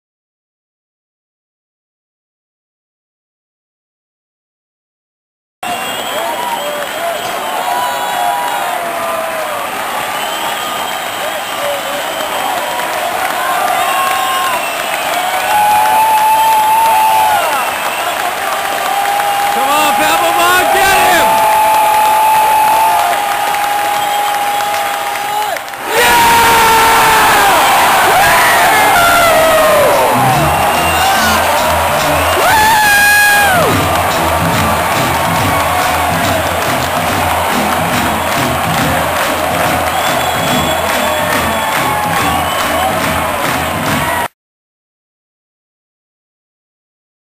Game 2 - World Series 2007- Papelbon closes it
Game 2.  World Series 2007.  Fenway Park.  Rockies v. Red Sox.  Papelbon gets his man.  Followed by The Troggs.